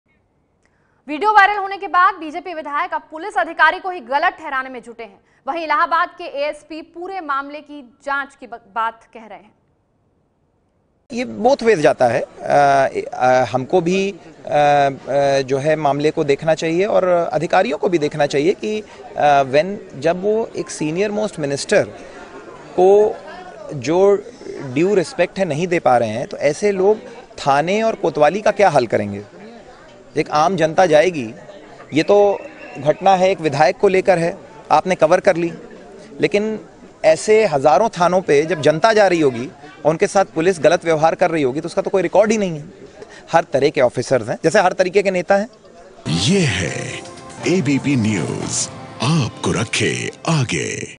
When They Can't Respect A Minister Think About Common Man, Says Harshvardhan On Threatening ASP
Uttar Pradesh's Deputy CM Keshav Prasad Maurya's aide Harshvardhan Vajpayee threatened ASP publicly. He has been accused of threatening to beat ASP up. The video is now going viral

Now, MLA Harshvardhan has come forth to speak about his own opinion about what happened. He said that if policemen would not respect a minister, how would they treat common people?